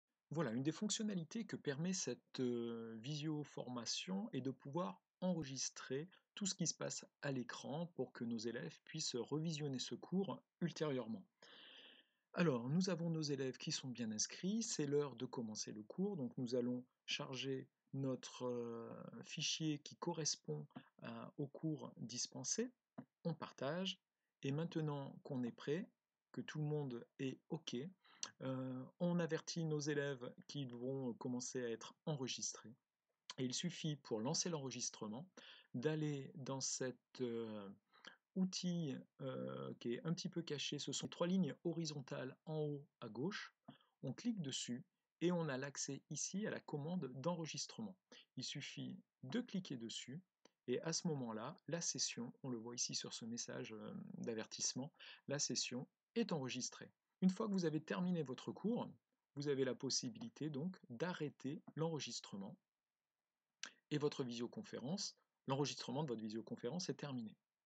L'enregistrement de la visio
Les élèves peuvent avoir envie de revoir la visioconférence ultérieurement, voilà comment faire pour l'enregistrer.